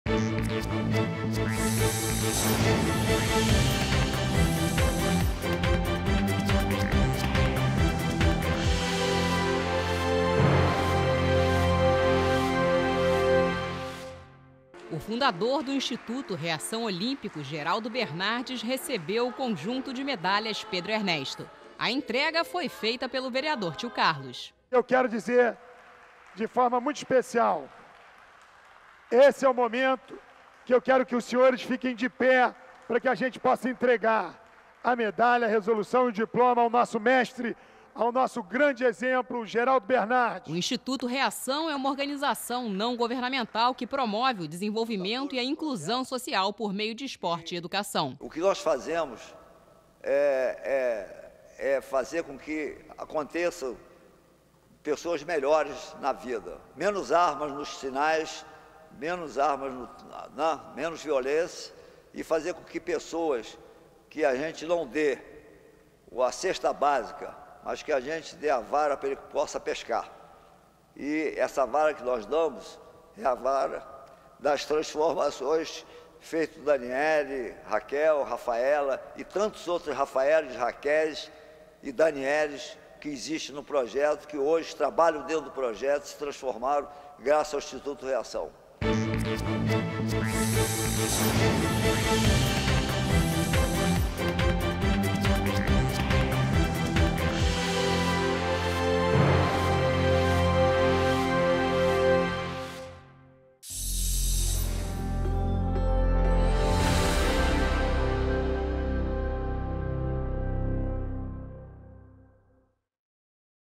Atividade Parlamentar - Edição 173 - 10.12.2014
O boletim Atividade Parlamentar traz as informações do que acontece no dia-a-dia da Câmara Municipal. Homenagens, atos públicos, exposições, CPIs e audiências públicas ganham divulgação imediata de acordo com sua importância.